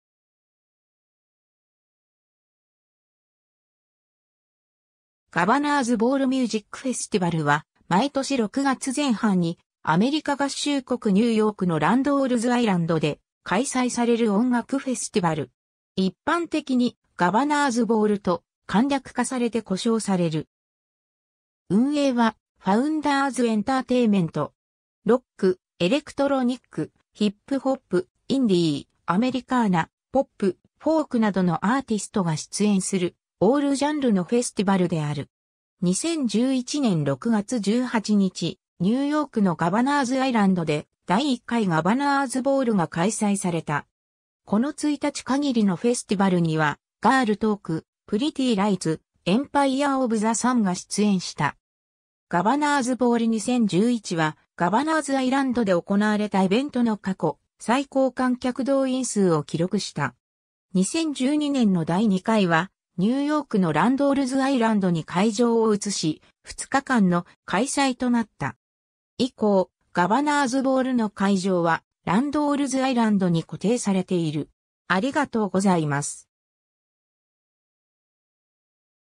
ガバナーズ・ボール・ミュージック・フェスティバル
ガバナーズ・ボール・ミュージック・フェスティバル（Governors Ball Music Festival）は毎年6月前半に、アメリカ合衆国ニューヨークのランドールズ・アイランドで開催される音楽フェスティバル。
一般的にガバナーズボール（Governors Ball）と簡略化されて呼称される。
運営はFounders Entertainment。
ロック、エレクトロニック、ヒップホップ、インディー、アメリカーナ、ポップ、フォークなどのアーティストが出演するオールジャンルのフェスティバルである。
 2011年6月18日、ニューヨークのガバナーズ・アイランドで第1回ガバナーズボールが開催された。
この一日限りのフェスティバルには、Girl Talk、Pretty Lights、Empire of the Sunが出演した。
ガバナーズボール2011は、ガバナーズアイランドで行なわれたイベントの過去最高観客動員数を記録した。
2012年の第２回はニューヨークのランドールズ・アイランドに会場を移し、2日間の開催となった。
以降、ガバナーズボールの会場はランドールズ・アイランドに固定されている。